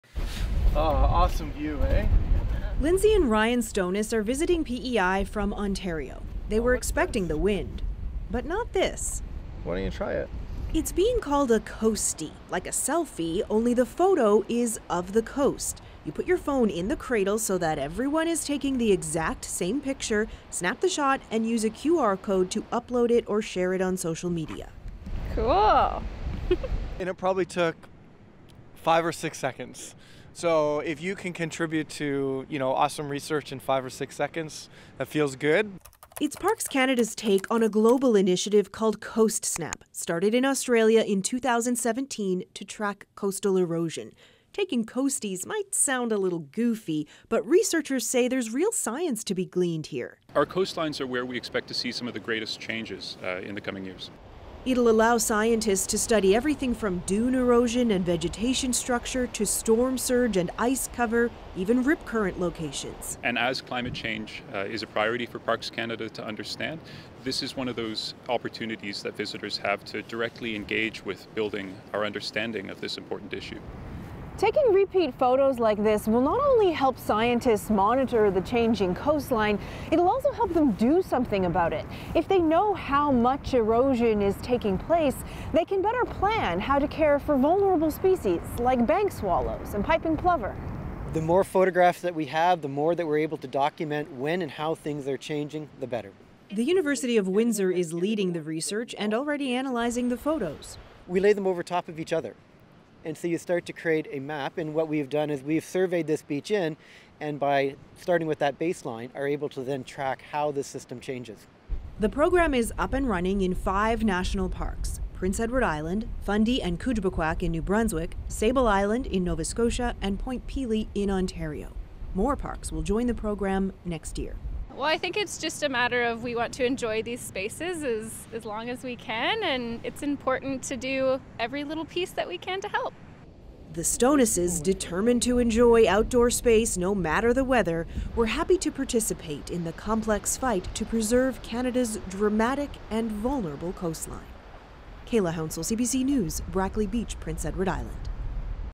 National park visitors asked to take photos to track climate change
Parks Canada has launched the Coastie Initiative in five national parks across Canada, encouraging tourists to take photos of the coastline that will help scientists monitor the impact of climate change.